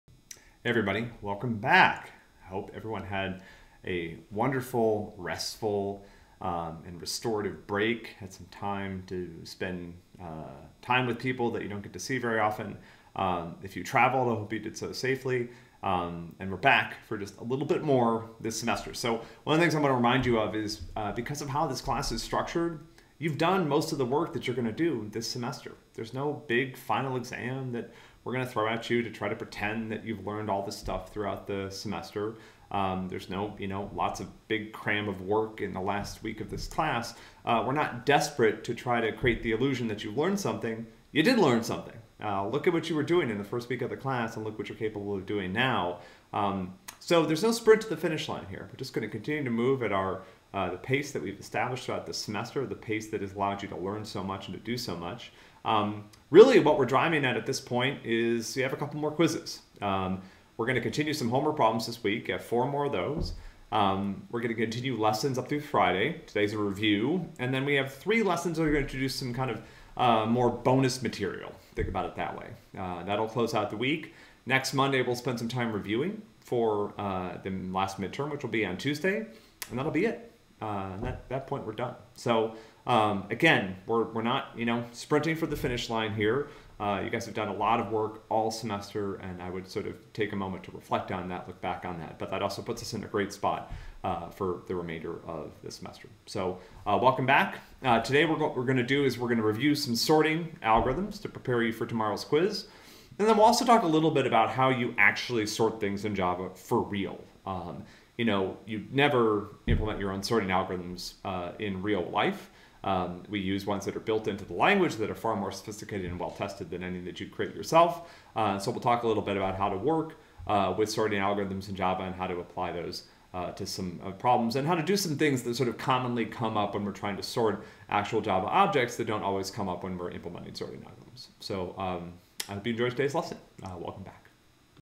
CS 125 Fall 2020 Lesson 60: Welcome Back!
Today we'll review sorting in preparation for tomorrow's quiz.